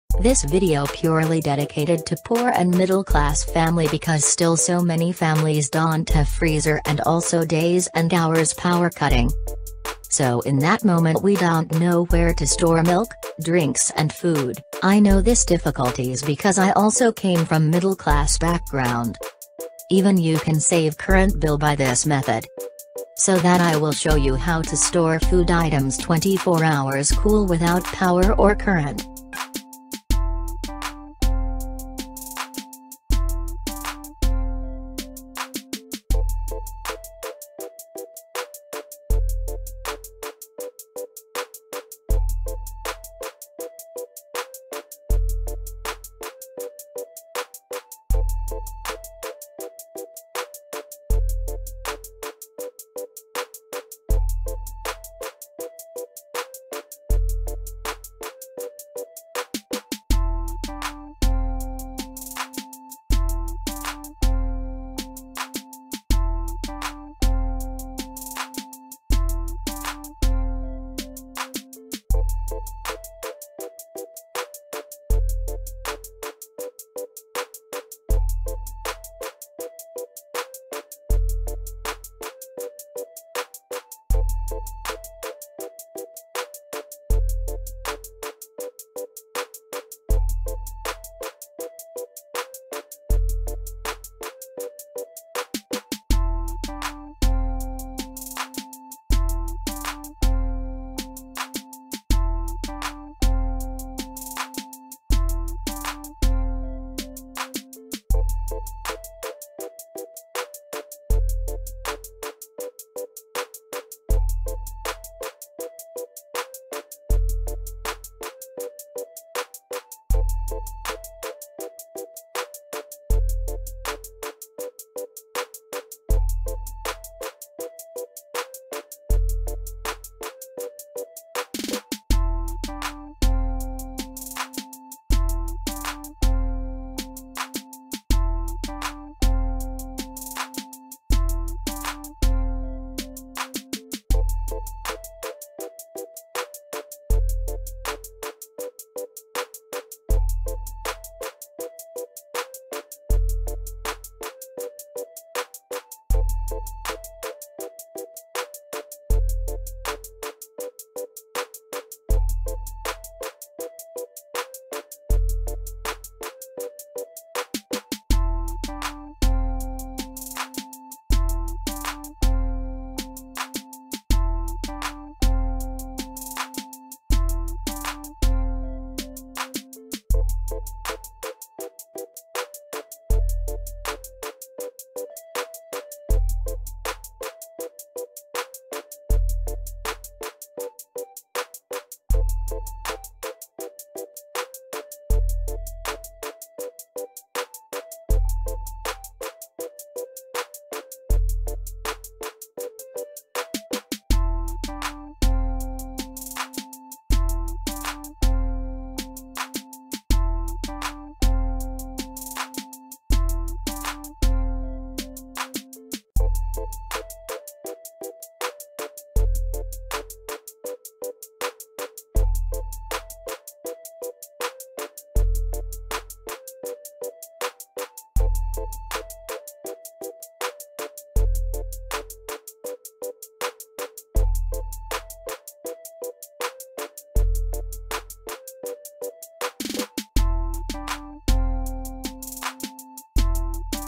Without Electricity and Without Machine How to make Refrigerator in 5-Minutes-( Low cost DIY)
this is summer hack will useful to poor and middle class families to survive without power and without current.